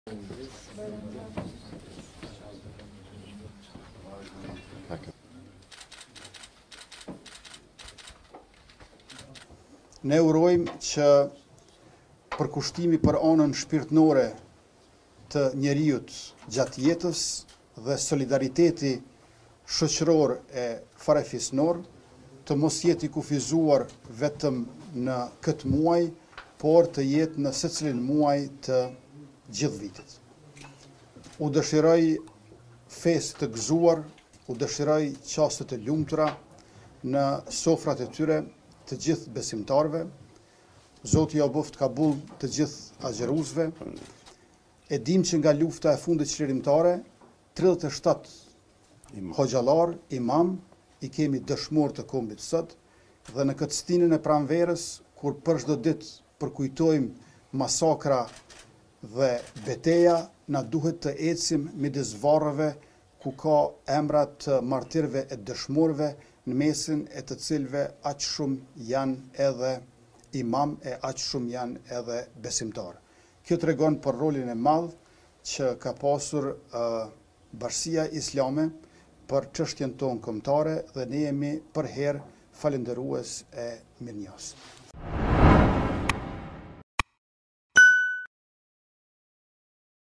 ​Kurti: BIK, rol të madh në çështjen kombëtare
Kryeministri Albin Kurti nga selia e Bashkësisë Islame të Kosovës (BIK) ka uruar të gjithë besimtarët myslimanë me rastin e Fitër Bajramit. Nga aty, kreu i ekzekutivit kujtoi edhe kontributin e BIK-ut në luftën për çlirimin e vendit.